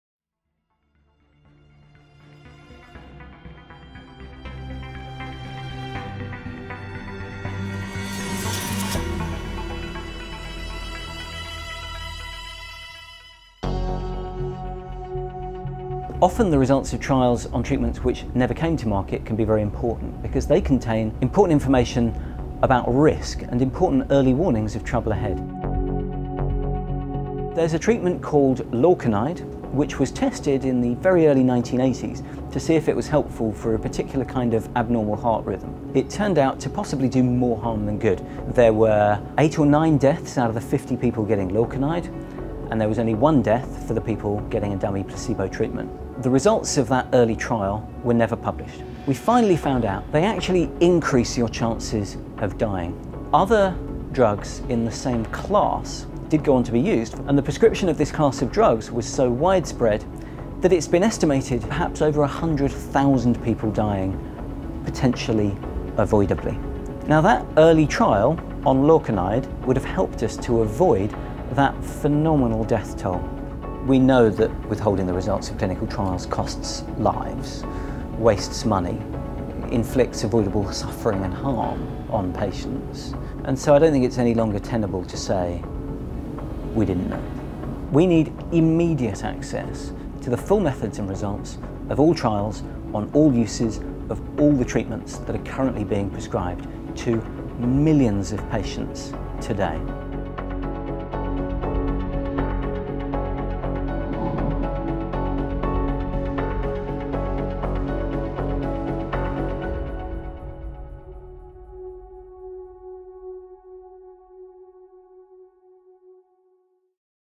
AllTrials: Withholding results costs lives
The results of a 1980 clinical trial on heart drug Lorcainide were not published until a decade later. Doctors didn't know that more people died in the trial who were given Lorcainide than who were taking the placebo. It has been estimated that over 100,000 people died avoidably because they were prescribed drugs in the same class.
Read about how the researchers were able to get the results of the study published

Join the AllTrials campaign to make all clinical trials count. Public pressure is already changing regulations and getting past trials published in some countries. You and everyone you know can join and by sharing this video.

This video was made from the interview with Dr Ben Goldacre during the filming of the AllTrials: Make clinical trials count video.

These videos were made possible by the donations of 600 individuals and RCVS Knowledge.

Created 2014